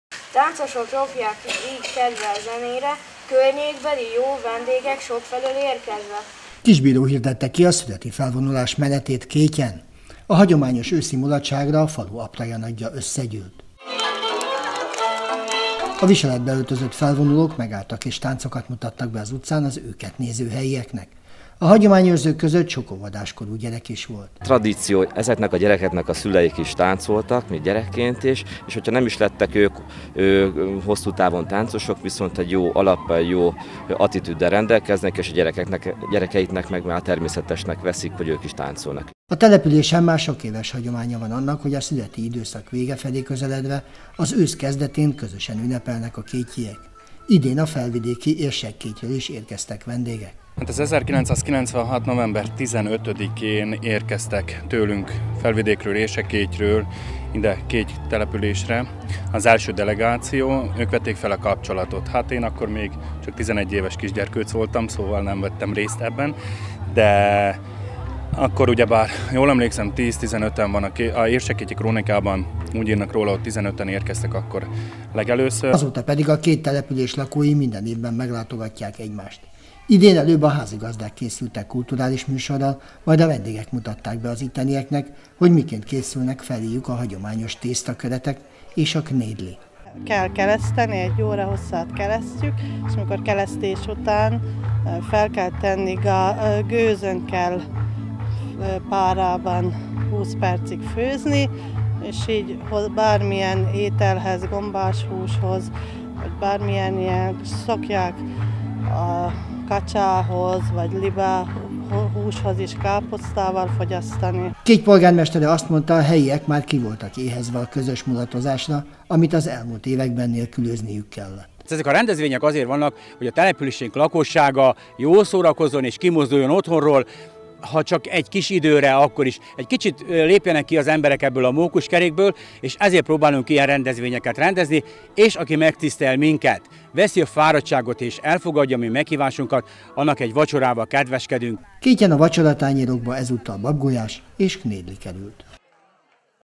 Szüreti mulatságot tartottak Kétyen - még Felvidékről is érkeztek vendégek
Szüreti mulatságot tartottak Kétyen - még Felvidékről is érkeztek vendégek a Tolna megyei településre

Igazán különleges módon ünnepelték meg Kétyen a szüreti időszak lezárását. A településen nem csak az ilyenkor hagyományos felvonulást tudták megtartani szombaton, hanem a környékbeli kulturális együtteseket is vendégül látták. Sőt, Felvidékről is érkeztek a faluba. A vendégek egy ottani finomság receptjét is megosztották a helyiekkel.